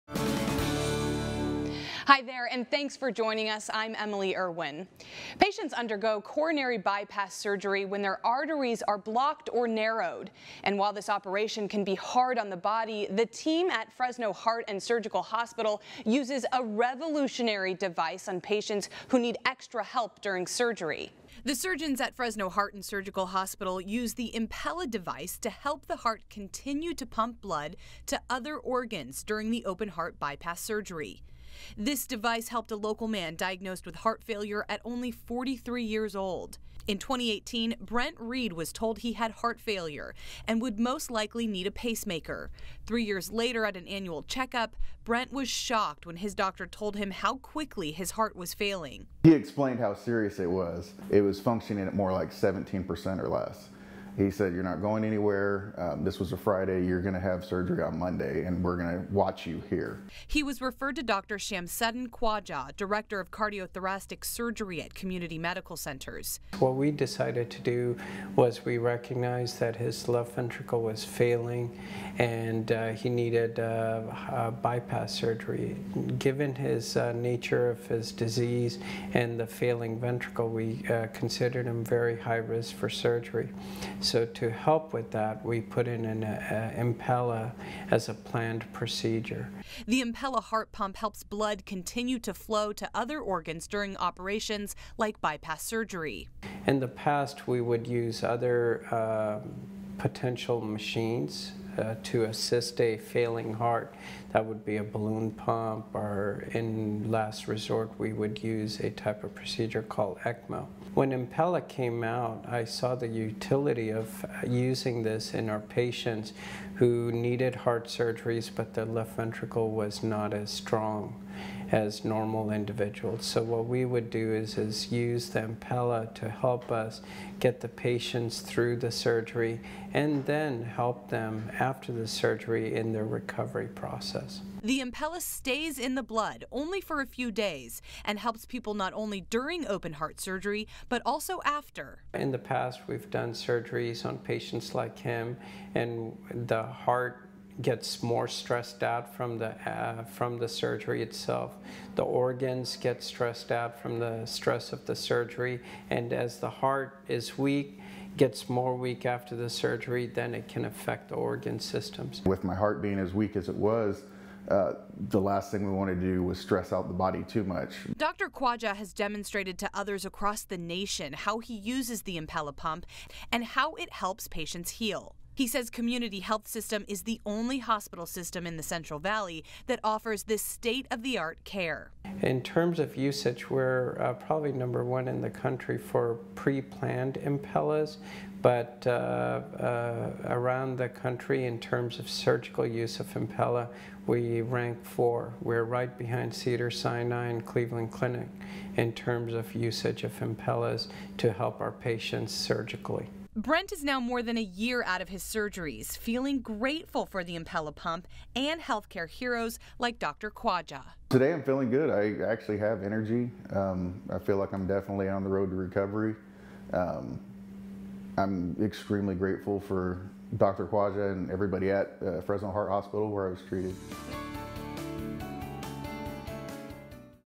Impella Pump Saves Man's Life
The Impella heart pump device is typically used on patients that suffer from severe heart failure. We sat down with Brent Reed who was told his heart was only operating at 17% to learn how Dr. Shamsuddin Khwaja, cardiothoracic surgeon with Community Medical Centers, used the Impella pump to help Brent get his life back.